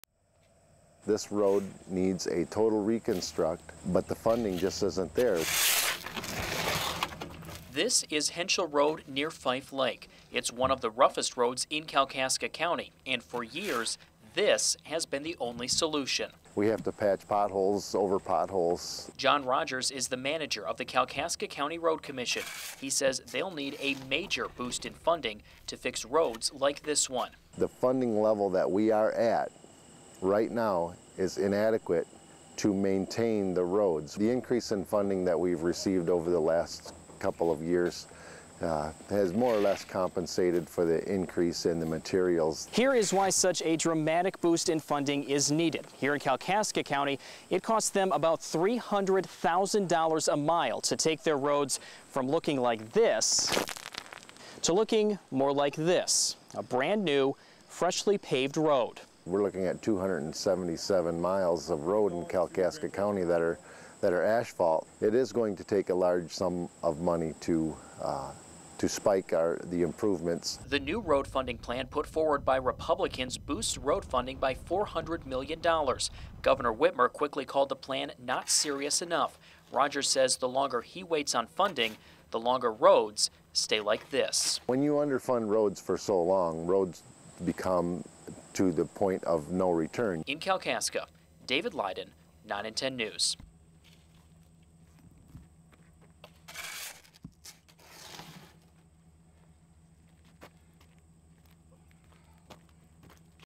Michigan Road Funding
Road commissions respond to road funding plan